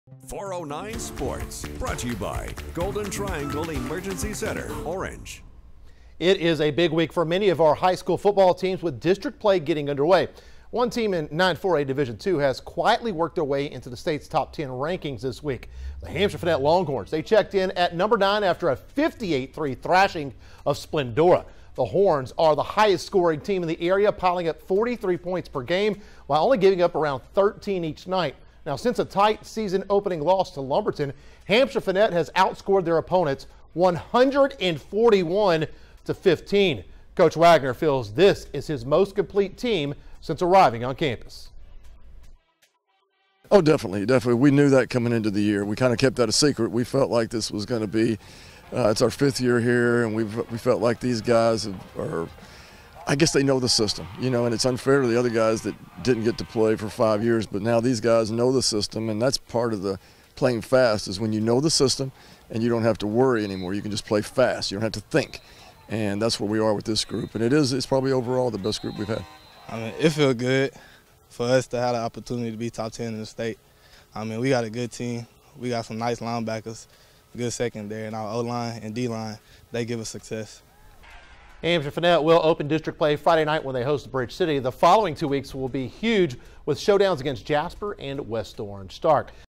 Hamshire-Fannett gains statewide attention after impressive start
Hamshire-Fannett is heating up just at the right time